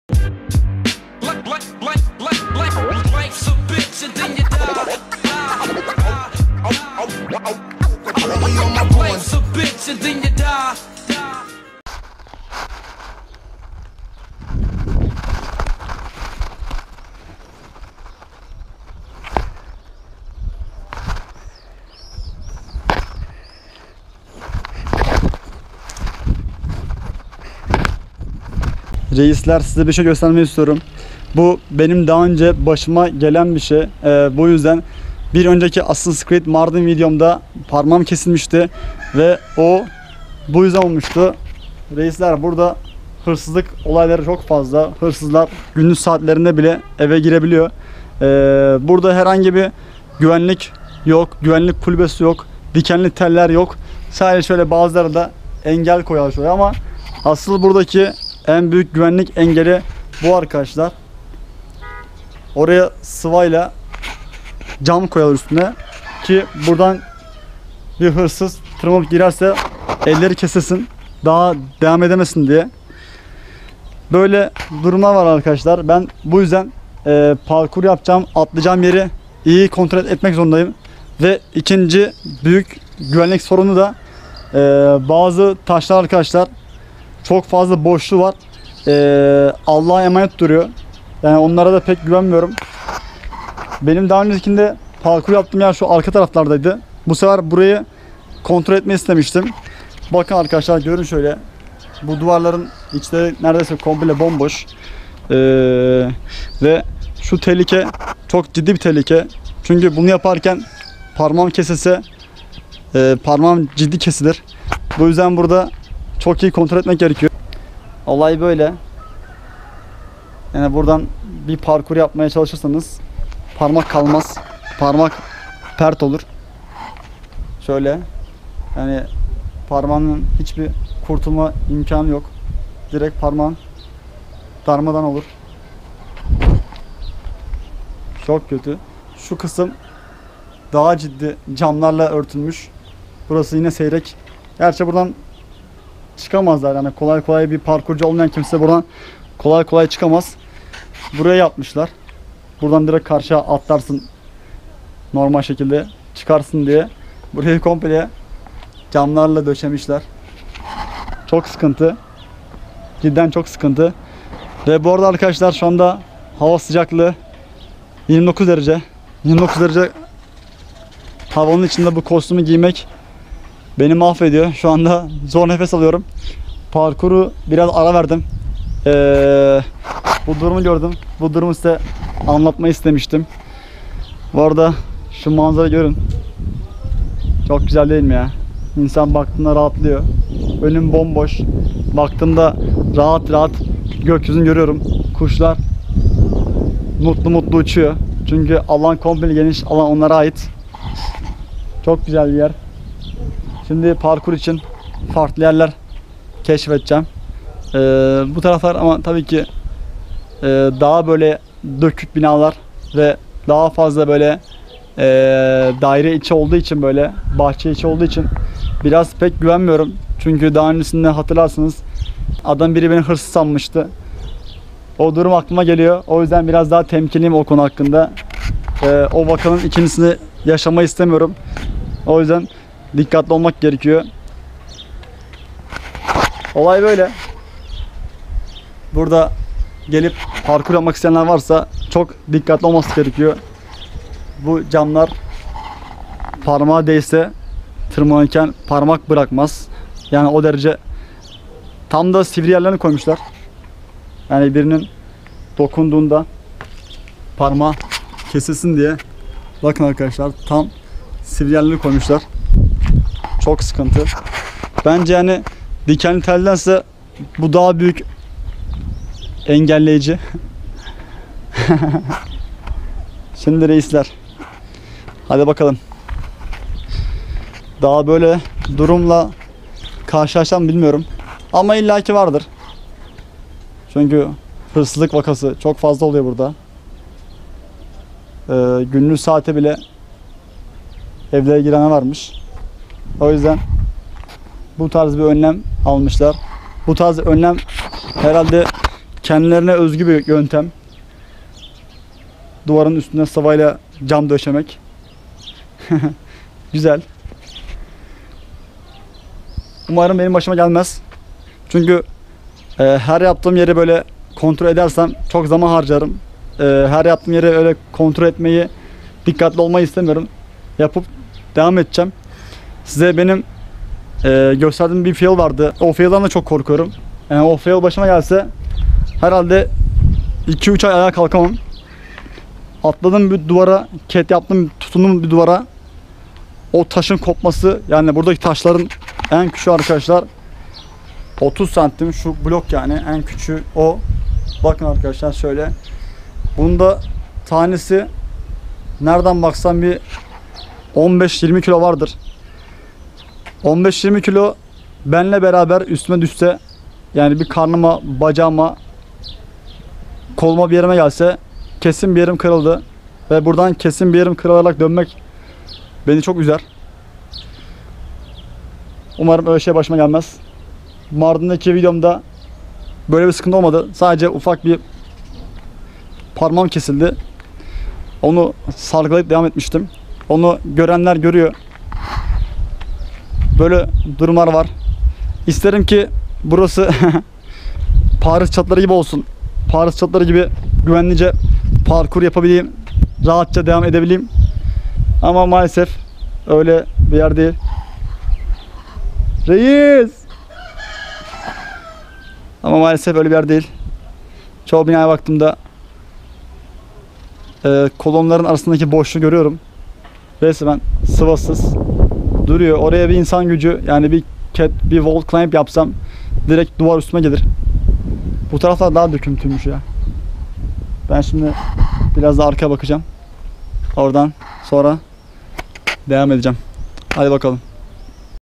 MARDİN'DE Kİ EVLERİN VE ÇATILARIN GÜVENLİK ÖNLEMİ Assassin's Creed Mardin
Reyizler herkese merhaba yine beraberiz yine sizlerleyim. 3 sene önce Mardin'de efsane Assassin's Creed Parkour'unu hazırlamıştım. Yıllar sonra tekrar özlediğim yerdeyim. Ülkemizde korona'nın en patladığı zamanda 17 günlük hapis moda 2 gün varken kısa zamanda gelip buraları Parkour ile tekrar değerlendirmek istemiştim ve Parkour esnasında daha önce elime yara veren bir durumu size göstermeyi istedim.